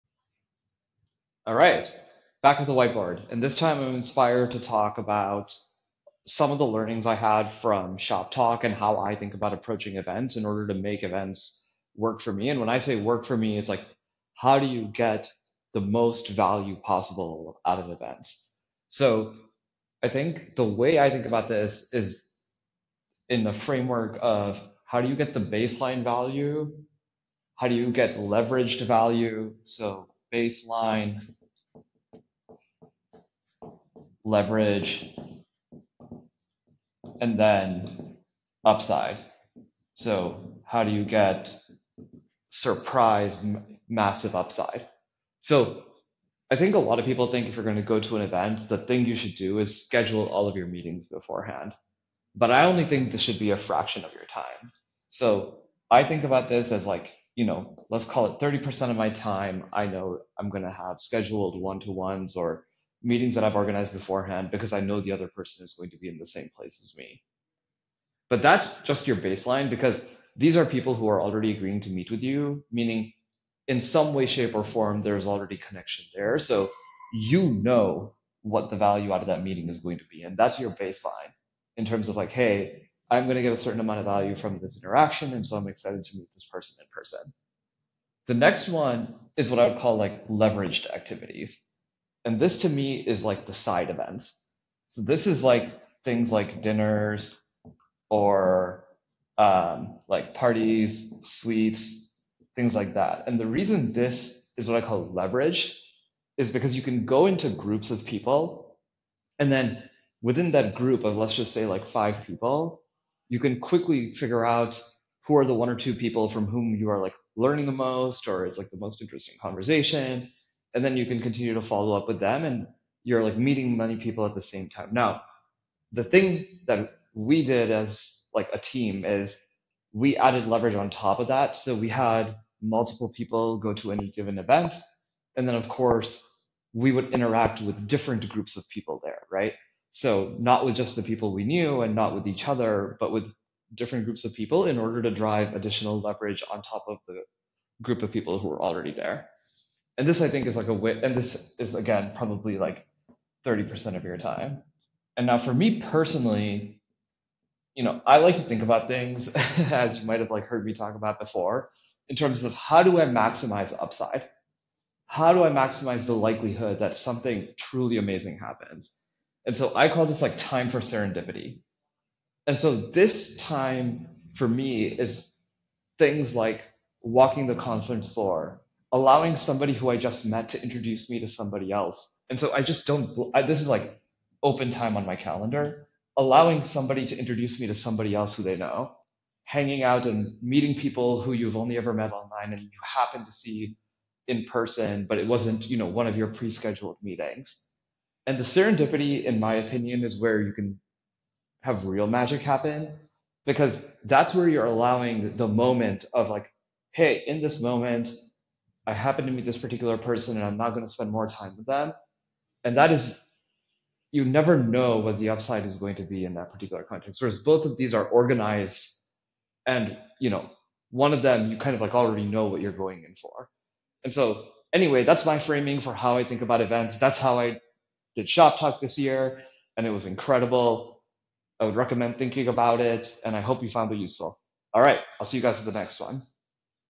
Making Events Work
👨‍🏫 Whiteboard Wednesday:

Here's how we think about making the events we host and sponsor work for FERMÀT